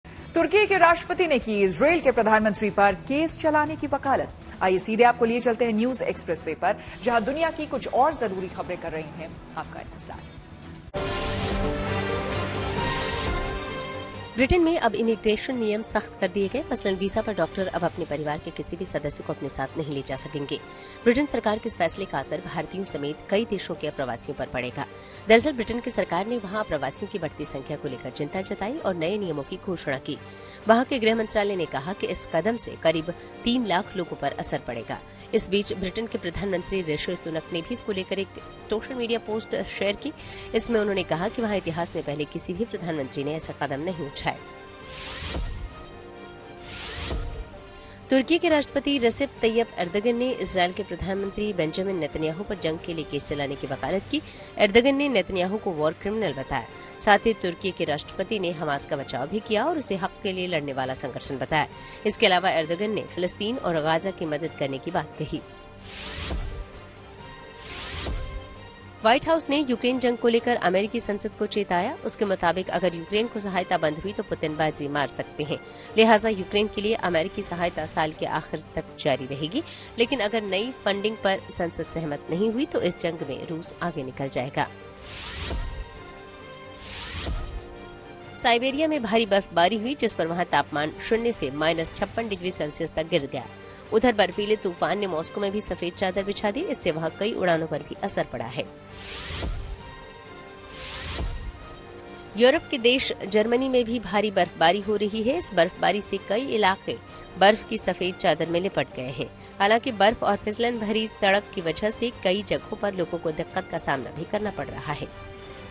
भारी बर्फ़बारी से Siberia में तापमान माइनस 56 डिग्री सेल्सियस तक,देखिए दुनियाभर की और भी खबरे
साइबेरिया में भारी बर्फ़बारी से तापमान शून्य से माइनस 56 डिग्री सेल्सियस तक गिर गया.  बर्फ़ीले तूफ़ान ने मॉस्को में भी सफ़ेद चादर बिछा दी. इससे वहां कई उड़ानों पर भी असर पड़ा है...